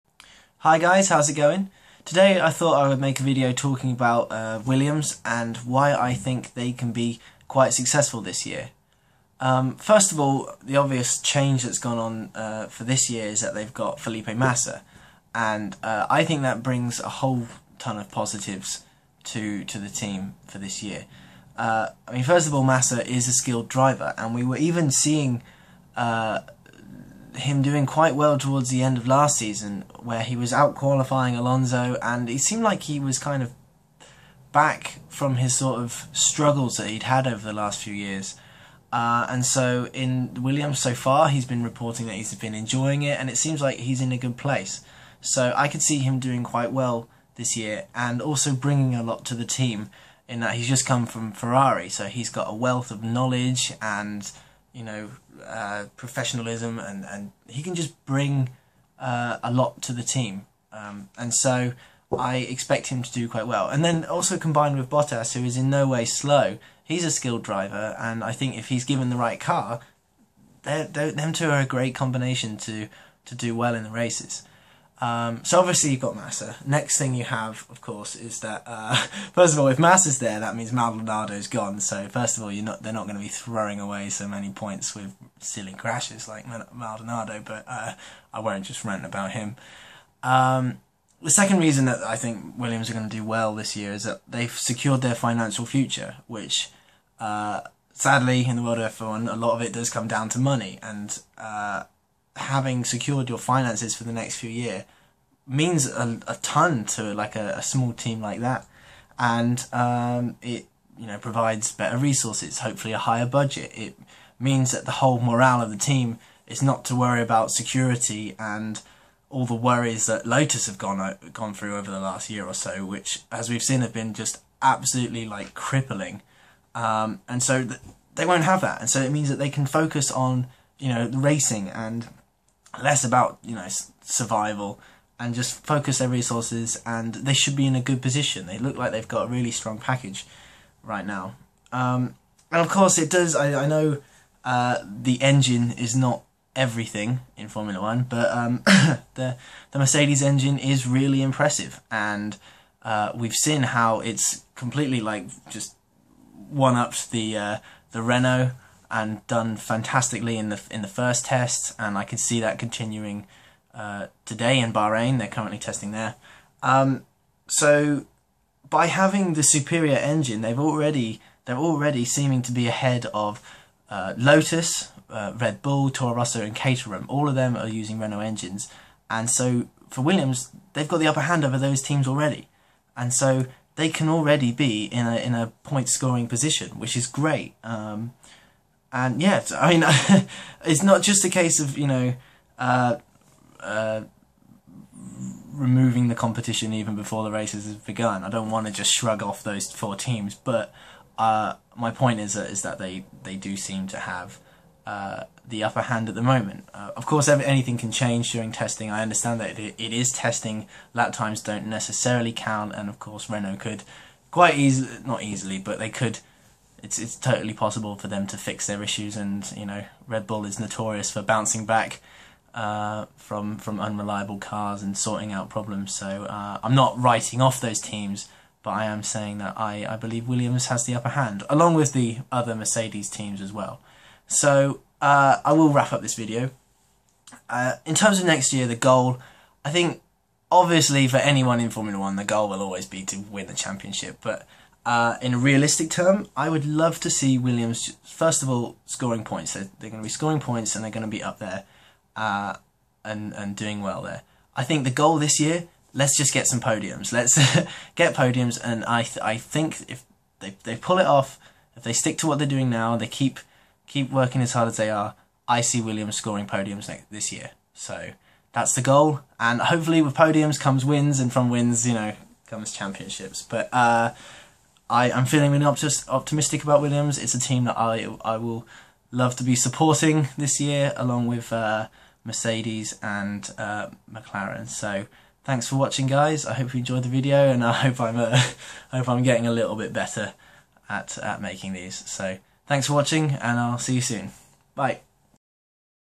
Williams in 2014!
My thoughts on the Williams F1 team in 2014. With Massa, and the new Merc Engine, the future looks good for Williams!